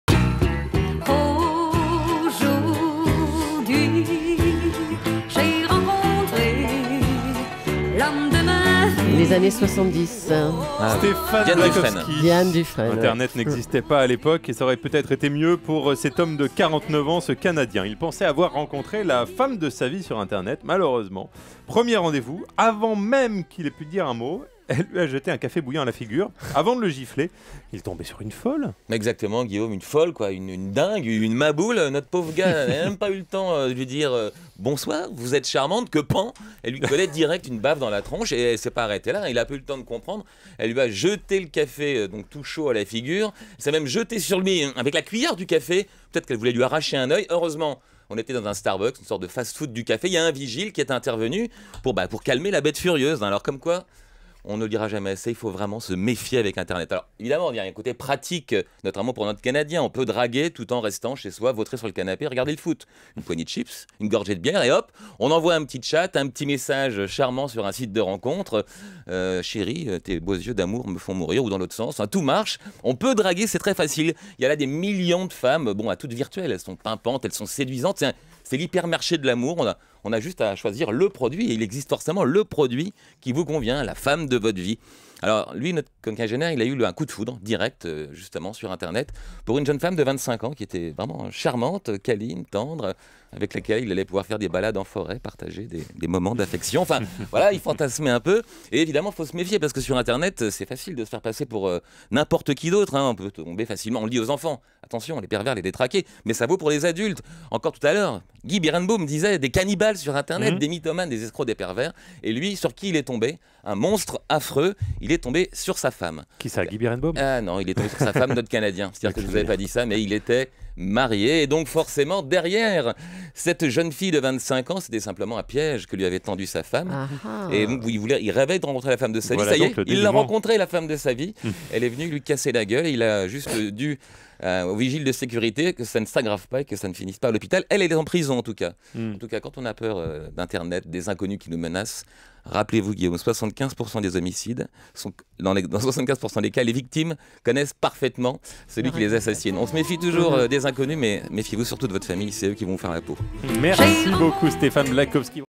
C'est moi ton rencard chéri !
Un homme s'est pris un café bouillant et une gifle par une femme qu'il venait de rencontrer sur le web.